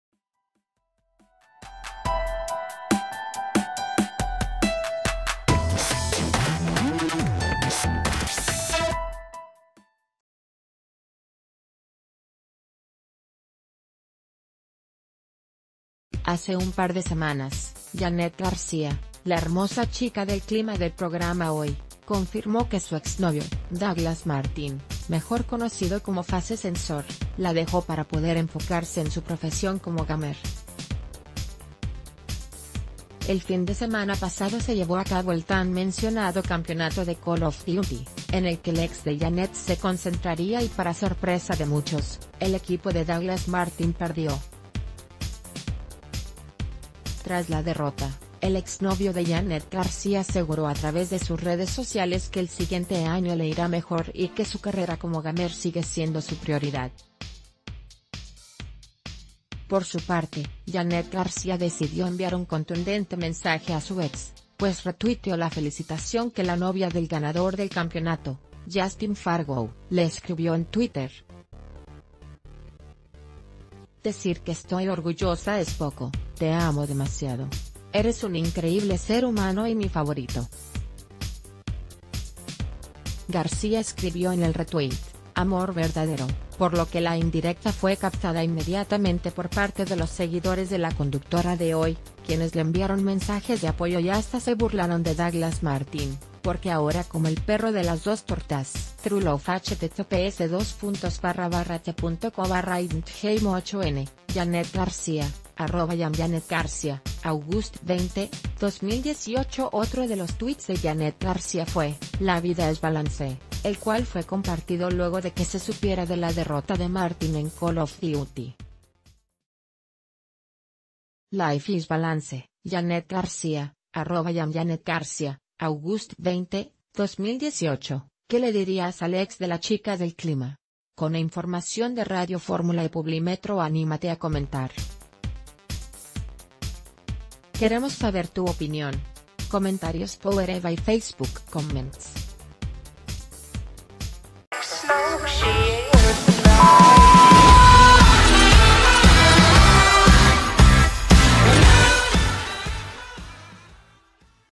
Yanet García reacciona tras derrota de su ex en campeonato de Call of Duty
Hace un par de semanas, Yanet García, la hermosa Chica del Clima del programa Hoy, confirmó que su exnovio, Douglas Martin, mejor conocido como FaZe Censor, la dejó para poder enfocarse en su profesión como gamer. El fin de semana pasado se llevó a cabo el tan mencionado campeonato de Call Of Duty, ...